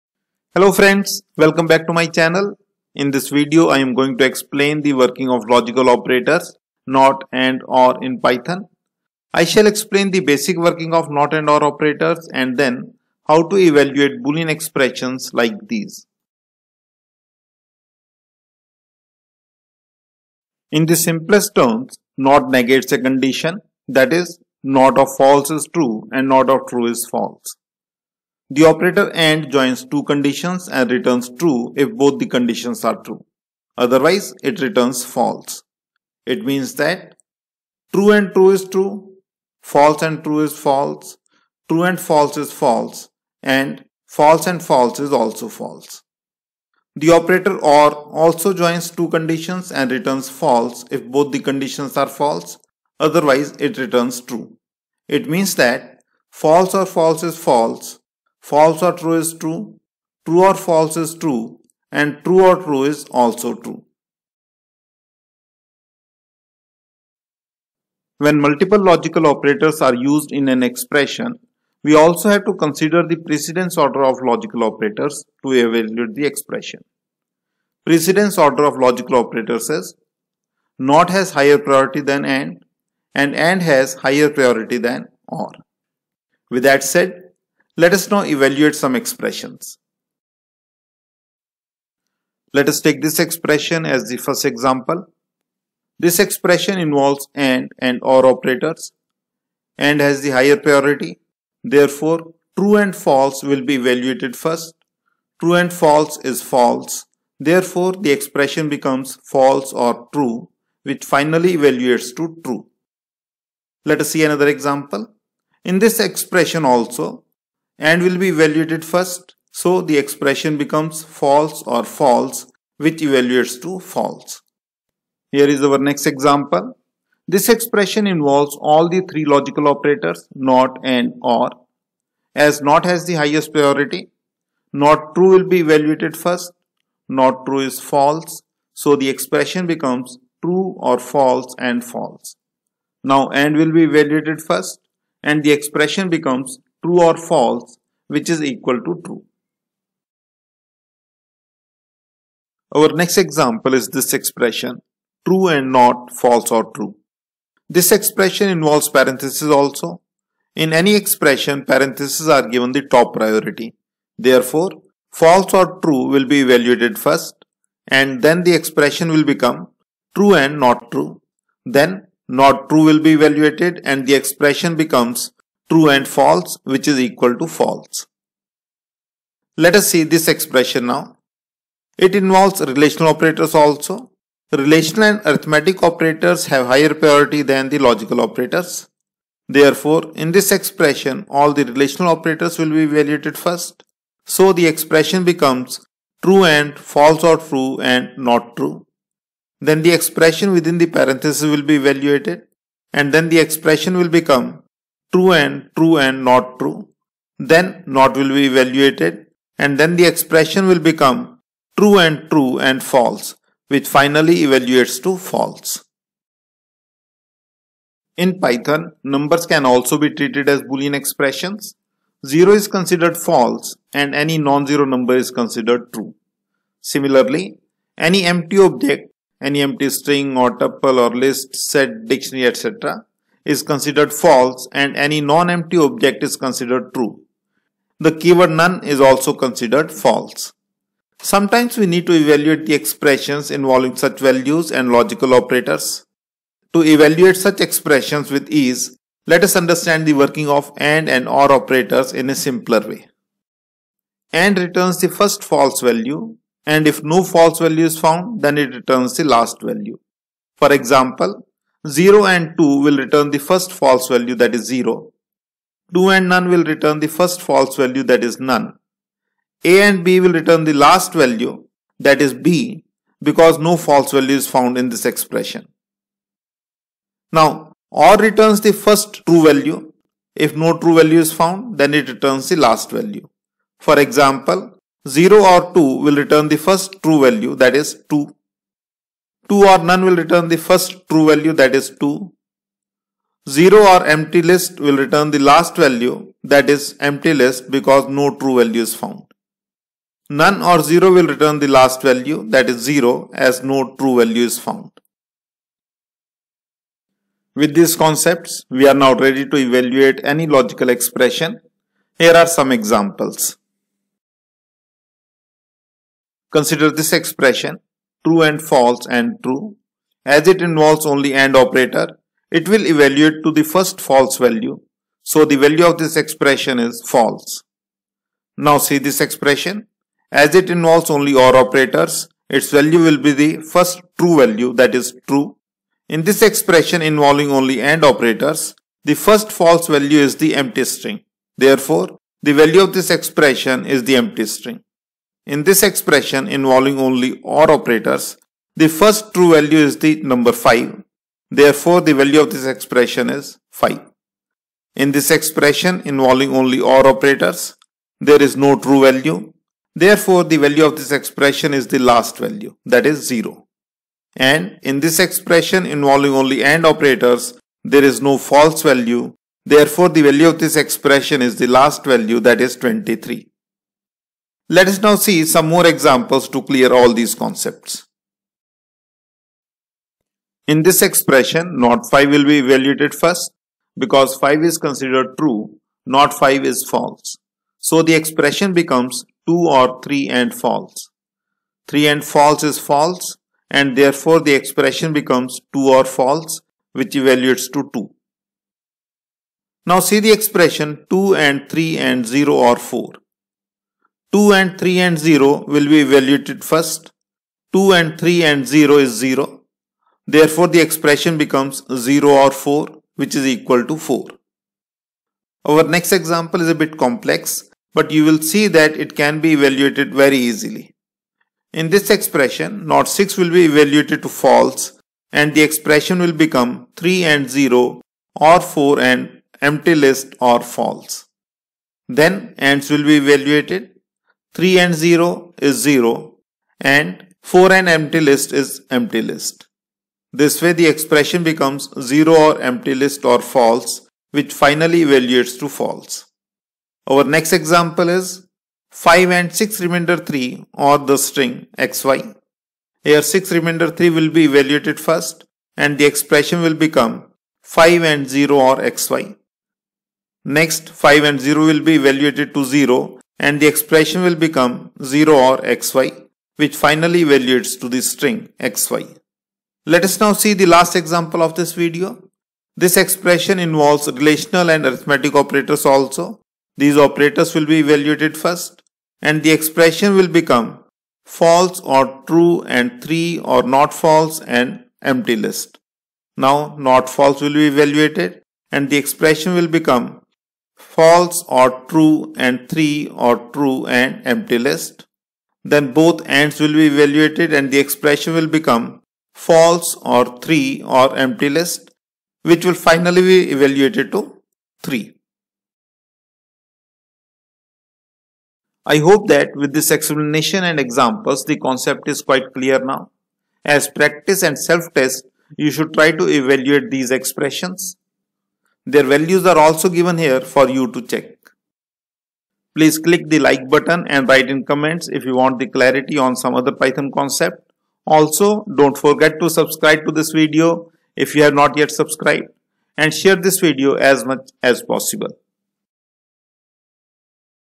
Logical Operators (not and or) and Boolean Expressions in Python
This video teaches how to evaluate the Boolean Expressions involving any kind of operands, like True, False, Numbers, Strings, Lists, Tuples etc.

Here one can learn to evaluate expressions like
True and False and True